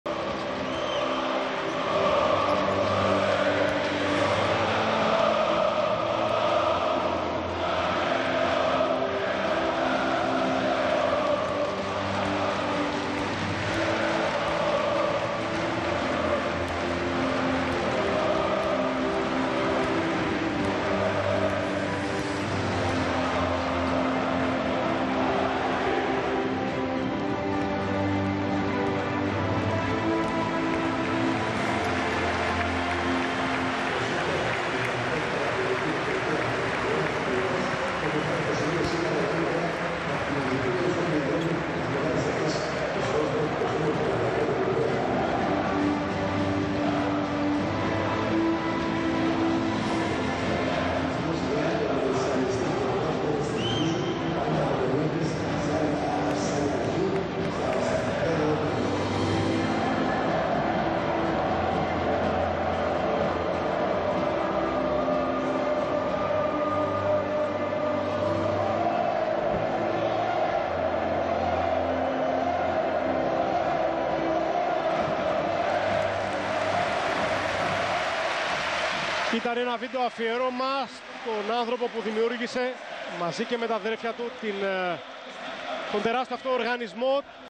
Βίντεο-αφιέρωμα προς τιμήν του αείμνηστου Παύλου Γιαννακόπουλου | 10/06/2024 | ΕΡΤ
Ολυμπιακός - Παναθηναϊκός | Basket League 2023-24 | Τελικός G3 | Ημέρα μνήμης για τον Παναθηναϊκό. Έξι χρόνια από την ημέρα που «έφυγε» ο Παύλος Γιαννακόπουλος.